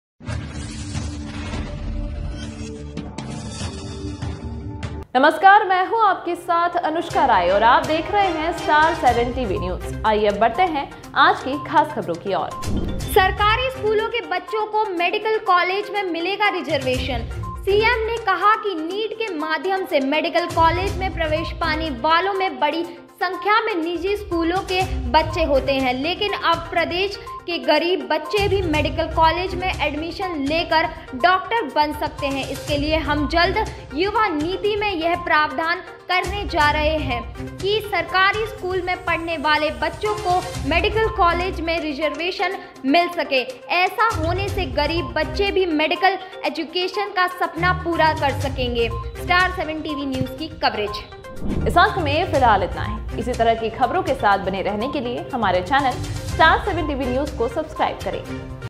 सीएम शिवराज की बड़ी घोषणा :सरकारी स्कूल के बच्चों को मेडिकल कॉलेज में मिलेगा रिजर्वेशन@Star7TVNews
ब्रेकिंग मध्यप्रदेश

सीएम शिवराज की बड़ी घोषणा :  सरकारी स्कूल के बच्चों को मेडिकल कॉलेज में मिलेगा रिजर्वेशन

सीएम ने कहा कि नीट के माध्यम से मेडिकल कॉलेज में प्रवेश पाने वालों में बड़ी संख्या में निजी स्कूलों के बच्चे होते हैं। लेकिन अब प्रदेश के गरीब बच्चे भी मेडिकल कालेज में एडमिशन लेकर डॉक्टर बन सकते हैं। इसके लिए हम जल्द युवा नीति में यह प्रावधान करने जा रहे हैं कि सरकारी स्कूल में पढ़ने वाले बच्चों को मेडिकल कॉलेज में रिजर्वेशन मिल सके। ऐसा होने से गरीब बच्चे भी मेडिकल एजुकेशन का सपना पूरा कर सकेंगे।

स्टार सेवन टीवी न्यूज़ की कवरेज@Star7TVNews